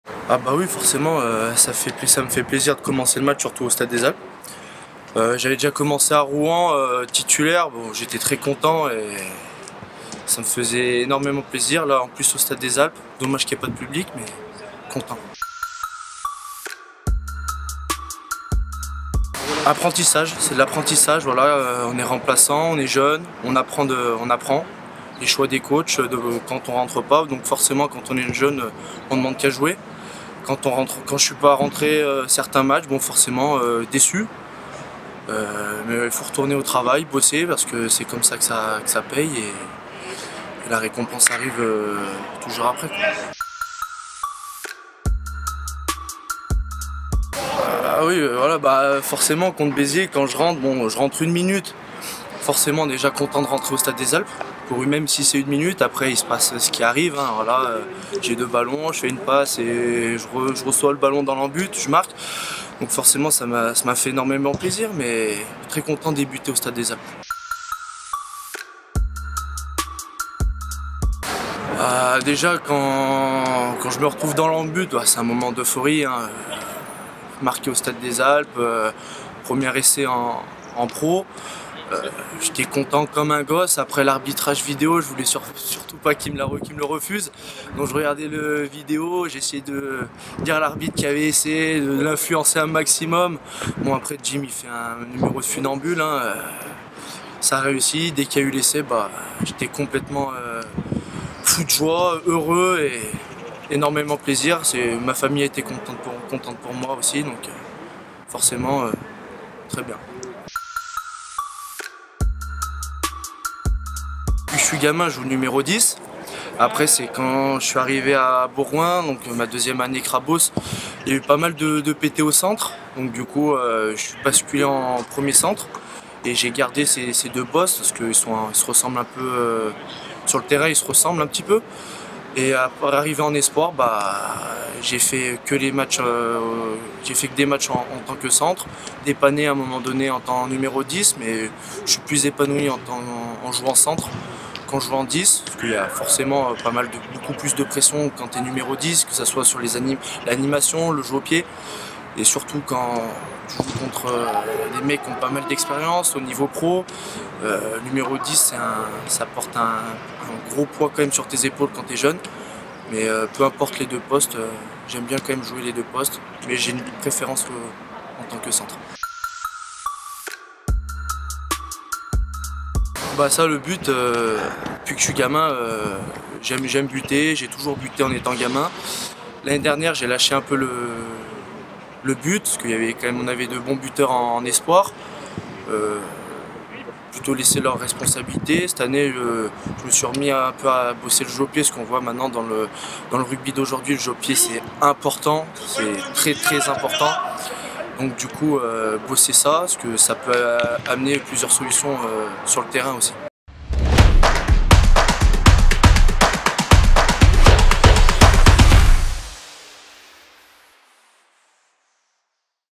Romain Trouilloud "j'étais content comme un gosse"
Romain Trouilloud, titulaire pour la rencontre face à Aurillac au Stade des Alpes nous livre son sentiment et reviens sur ces derniers matchs.